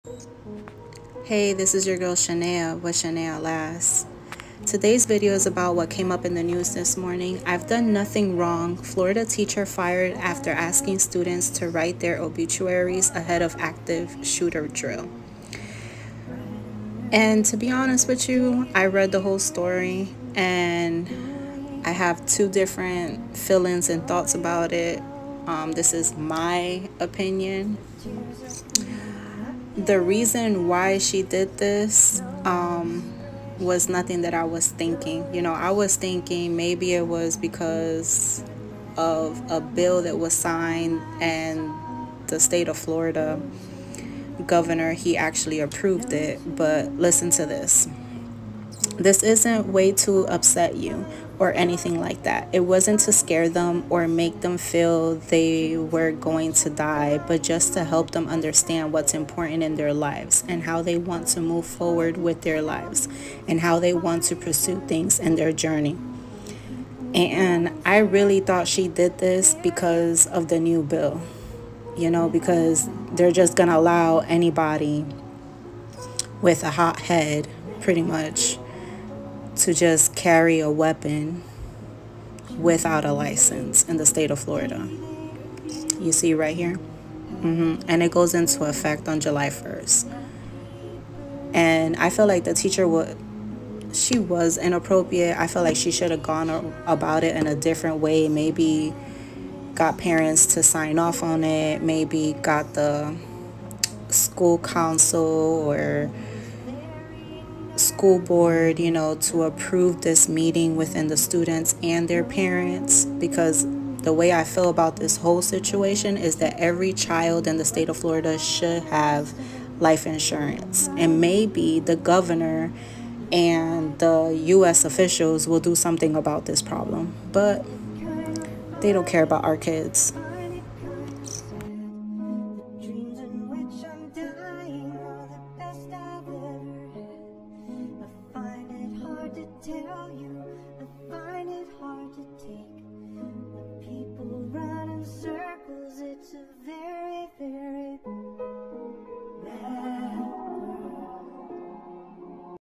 Teacher fired????
Teacher fired after making students write their obituaries before active shooter drill.